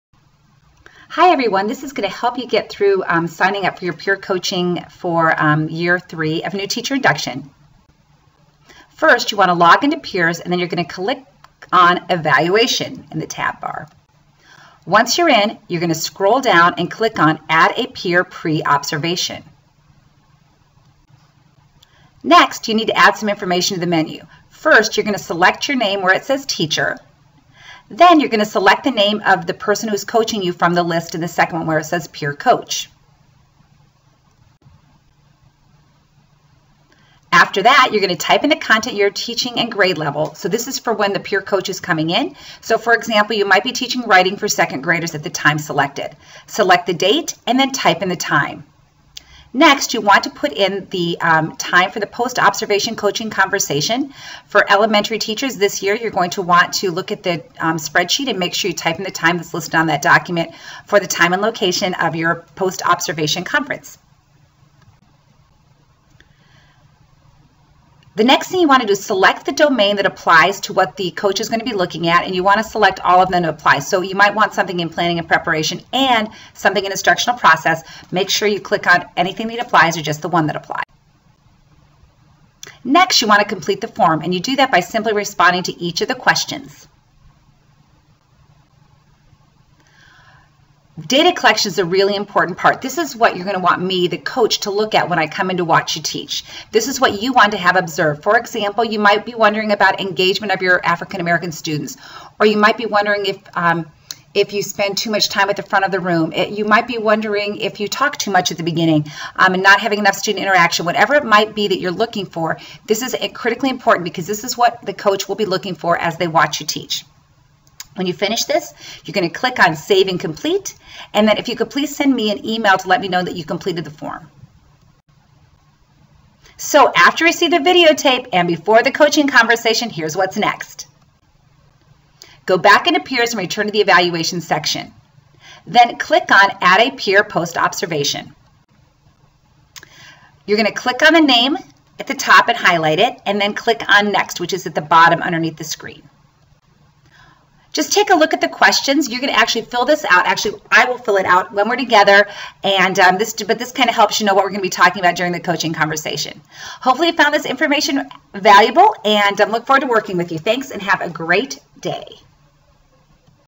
Coordinator Observation Year 3
A "how to" video for utilizing the Peer Coaching section of the Evaluation in PEERS/